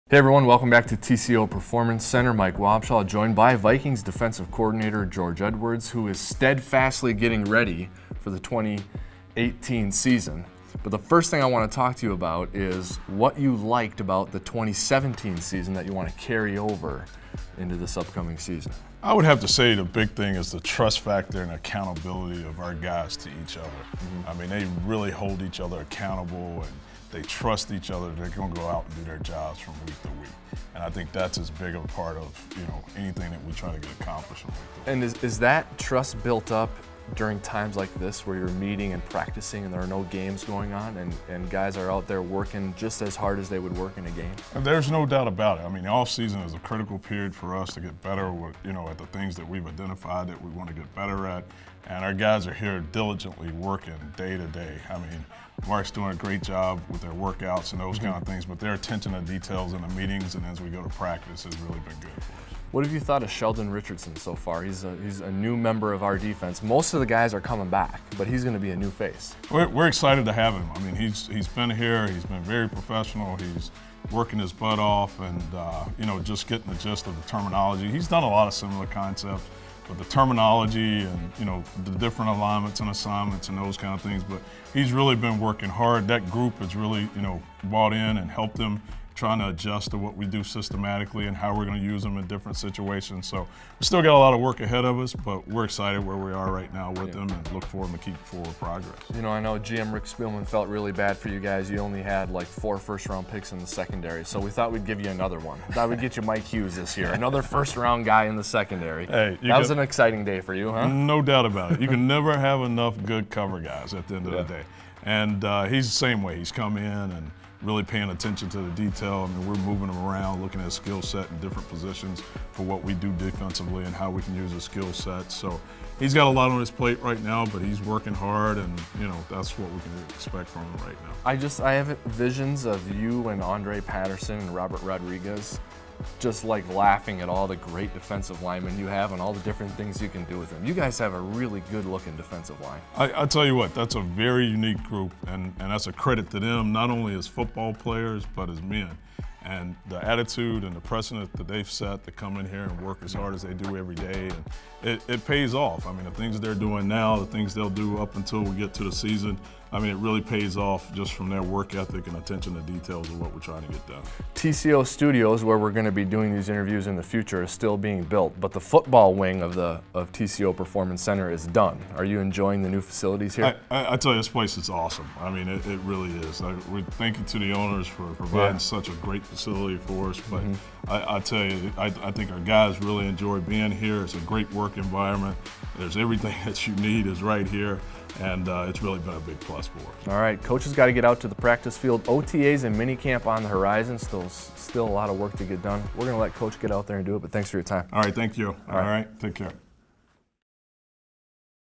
George Edwards on Sheldon Richardson Signing, Mike Hughes Pick, More
Mike Wobschall was joined by Vikings Defensive Coordinator George Edwards to reflect back on how things went in 2017, and then to look ahead to 2018, including the addition of DT Sheldon Richardson, first round pick CB Mike Hughes, and more.